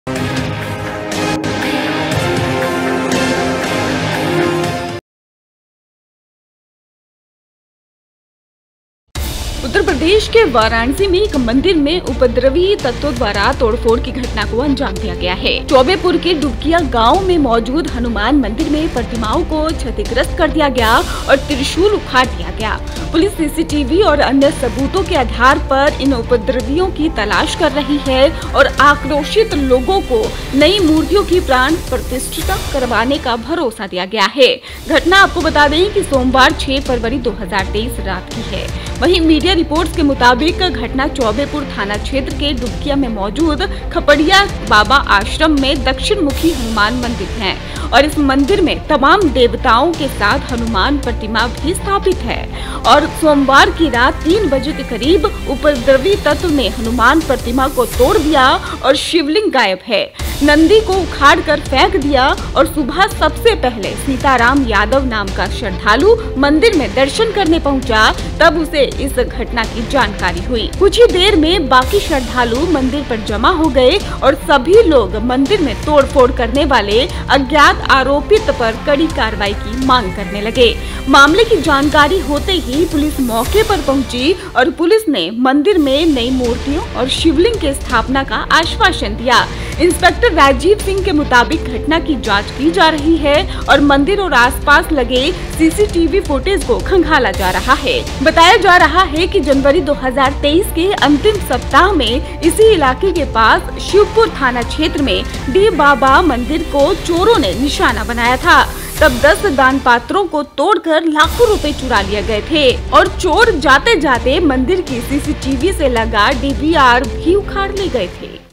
वाराणसी के मंदिर में तोड़फोड़ से लोग आक्रोशित..Police ने दिलाया ये भरोसा | UP NEWS
Incident of vandalism by miscreants has come to light in a temple in Varanasi, Uttar Pradesh. The idols were damaged in the Hanuman temple located in Dubkiyan village of Chaubepur. Trishul was uprooted. Police is looking for miscreants on the basis of CCTV and other evidence.